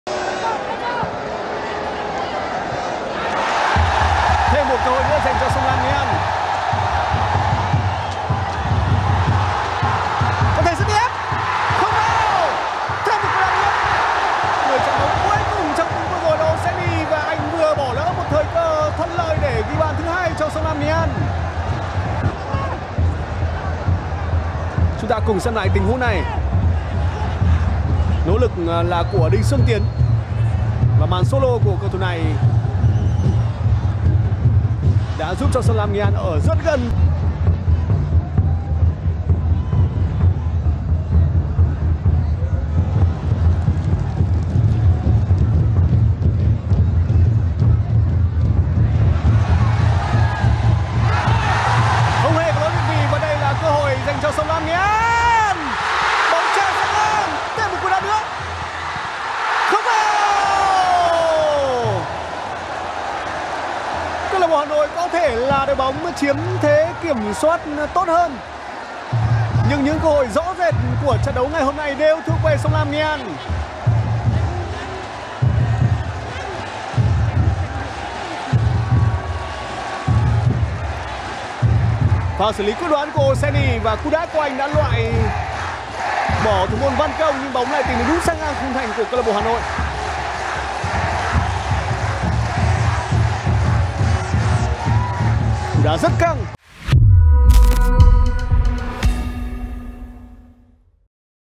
2 pha bỏ lỡ của Oseni và sự nuối tiếc của SLNA khi không thể giành thắng lợi trước Hà Nội tại Vinh
VPF Media | 2 pha bỏ lỡ của Oseni và sự nuối tiếc của SLNA khi không thể giành thắng lợi trước Hà Nội tại Vinh